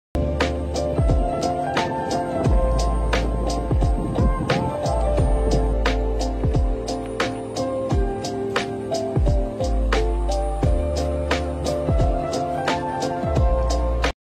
Yarbo in Harbin: Snow Clearing in the Radiant Sunset Glow
Join us in the magical city of Harbin as Yarbo effortlessly conquers the snowy landscape, painting a picturesque scene in the enchanting glow of the sunset. Experience the beauty of technology meeting nature as we redefine winter yard care with grace and innovation. ❄️✨